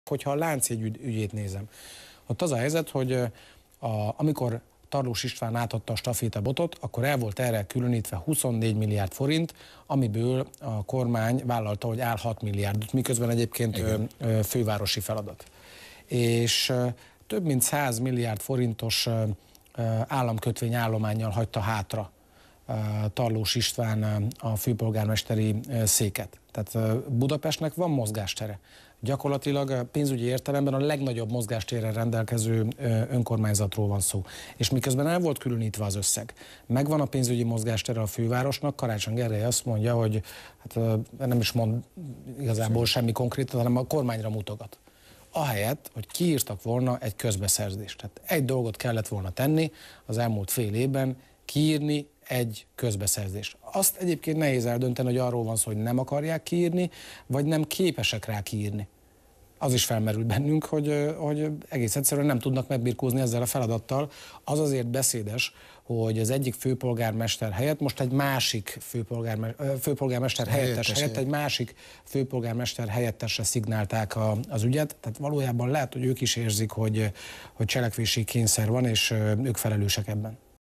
Dömötör Csaba a Lánchídról
Budapestnek megvan a pénzügyi mozgástere a Lánchíd felújításához. Egy dolgot kellett volna tenni, kiírni egy közbeszerzést. Az is felmerült bennünk, hogy nem képesek megbirkózni ezzel a feladattal.